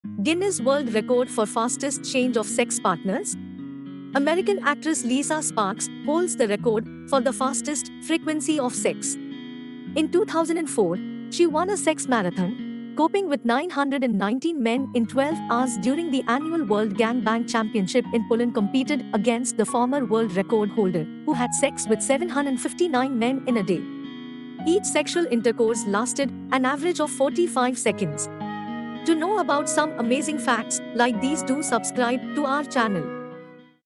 Sex Marathon | Sex Olympics | Sex Olympic games | sex Olympic game
This video will introduce you to some Amazing facts & Information about "Lisa Sparks" & Sex Marathon" "Sex Olympics".

A unique sex record / Sex Olymics / Sex marathon by Liza Sparks.

All information & Facts shared in the video are gathered from Online resources.